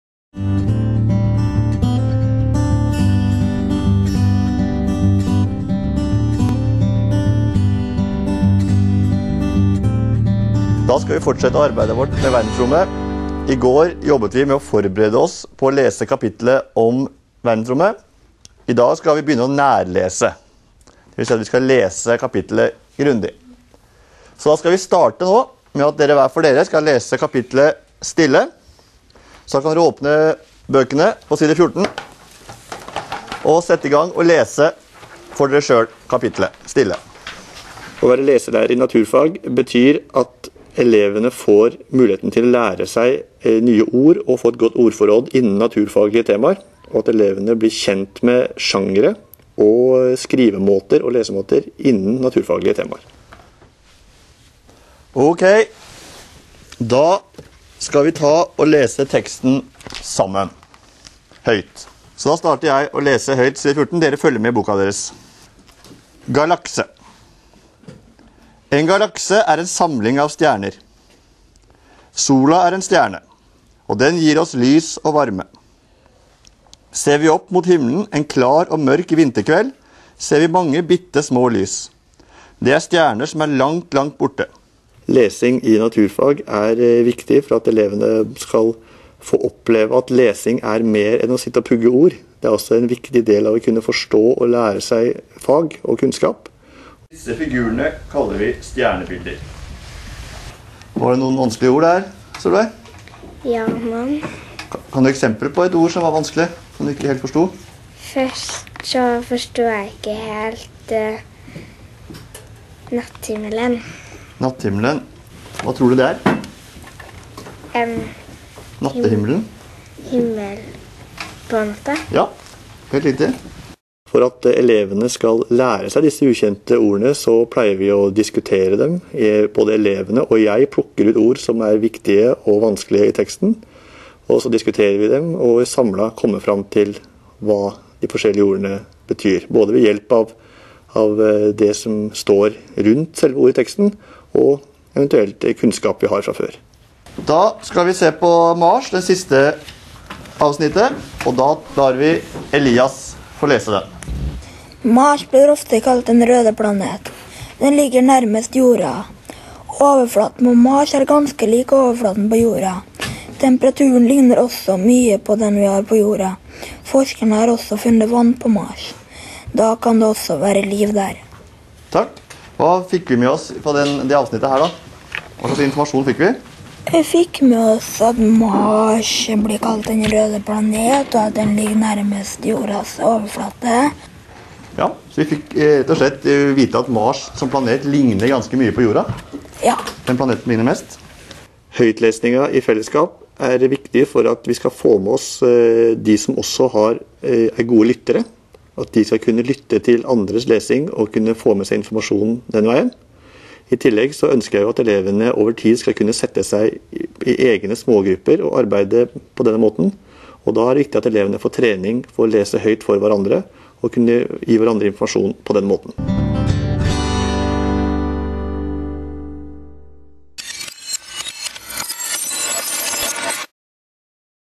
Lesing og skriving i naturfag: lesing
Filmen viser et helhetlig undervisningsopplegg for 4. trinn ved Tanem oppvekstsenter i Klæbu kommune. Undervisningsopplegget går over tre dager, og filmen viser hvordan lærer Fredrik Gustavson tar elevene gjennom førlesingsaktiviteter, lesing, forberedelser til skriving og det ender opp med skriving av en fagtekst.

Temaet elevene jobber med er verdensrommet. Dette er et tema elevene kan mye om fra før. Førlesningsaktivitetene begynner derfor med å aktivere noe av denne forkunnskapen gjennom en brainstorming. Etterpå leser elevene fagteksten med BISON-blikk. Det gir en mulighet for å innlæring av nye begreper som legger grunnlaget for en bedre forståelse når elevene skal lese teksten.

I selve lesingen av fagteksten viser filmen hvordan man kan variere leseaktivitetene slik at flest mulig elever får med seg innholdet. Vi ser hvordan elevene leser teksten individuelt, hvordan læreren leser høyt for elevene og hvordan elevene leser høyt for hverandre; alt supplert med plenumsdiskusjoner og forklaring av fagbegreper.

Den tredje sekvensen viser hvordan klassen forbereder seg til å skrive en fagtekst om temaet. Læreren har laget en oppgave der elevene får bruke fagstoffet de har arbeidet med. I tillegg får elevene kriterier for hvordan teksten skal være, og de får oppgitt hvem de skal skrive til. For å gi ytterligere støtte til fagskrivingen lager klassen i fellesskap et strukturert tankekart over temaet verdensrommet.

I den siste sekvensen ser vi hvordan elevene skriver selve fagteksten. Den viser hvordan begynner med å skrive hoveddelen i fagteksten, og at de først etter å ha skrevet hoveddelen skriver innledning og avslutning ved hjelp av plenumsstyrt undervisning. I tillegg gir elevene respons på tekstene til hverandre ut fra to forhåndsgitte spørsmål.

Spørsmål til diskusjon
1. Hva er bra med dette undervisningsopplegget?
2. Hvilke fordeler er det med å ta i bruk flere ulike førlesningsaktiviteter?
3. Hva oppnår elevene ved å bruke BISON-blikk på fagteksten?
4. På hvilken måte fungerer tankekartet som støtte for skriving av fagtekst i naturfag?
5. Hva oppnår elevene ved å gi respons til hverandre?